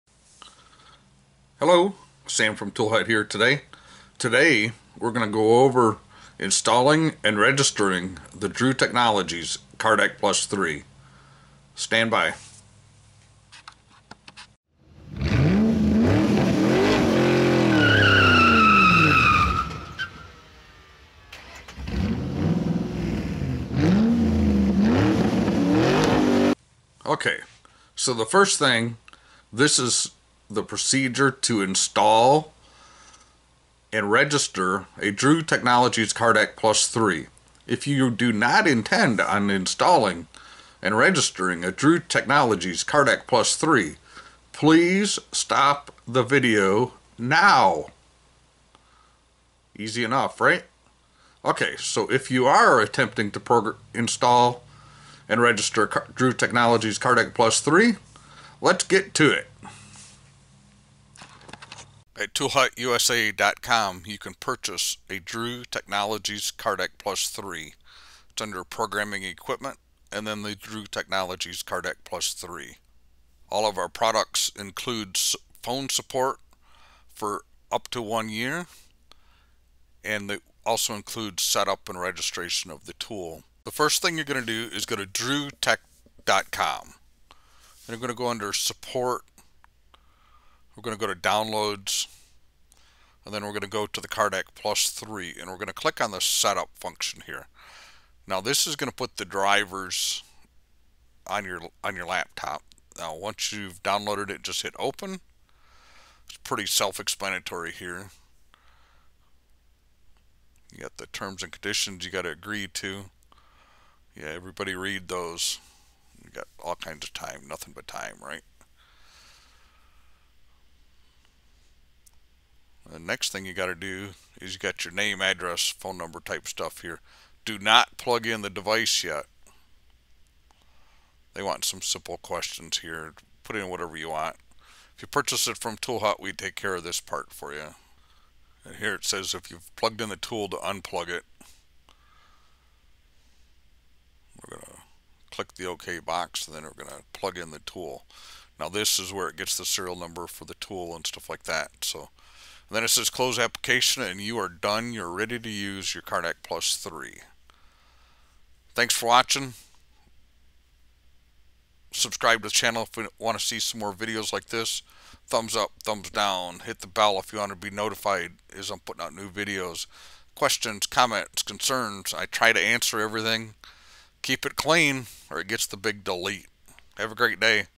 Installing and registering Drew cardaq plus 3
We are going through the process to install and register your drew technologies cardaq plus 3.

Want a ToolHutUSA sticker send $5 USD specify it is for a sticker. Outside US please add some extra for additional postage.